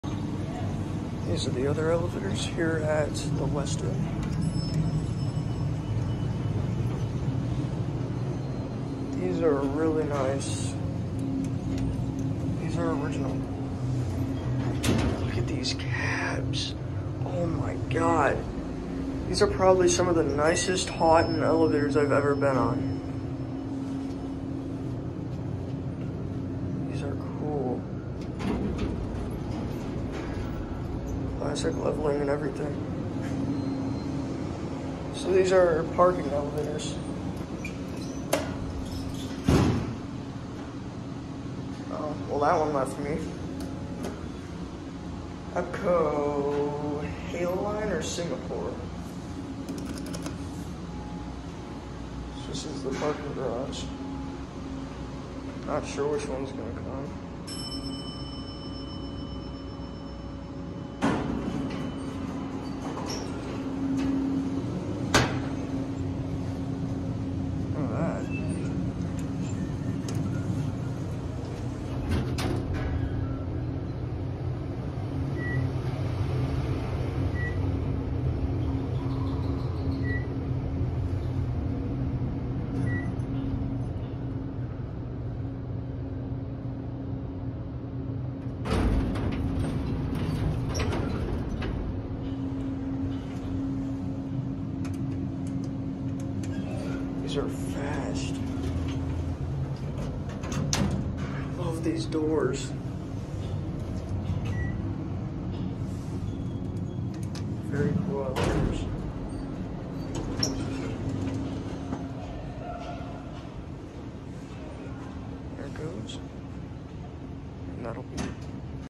Awesome Original Schindler Haughton Traction Parking Elevators | Westin | Pittsburgh, PA
(4/4/24)
YOOO!! These are awesome!! Original Haughton bottom drive tractions that haven’t been touched since installation. Hopefully these stay original for a while longer.

Installed: 1984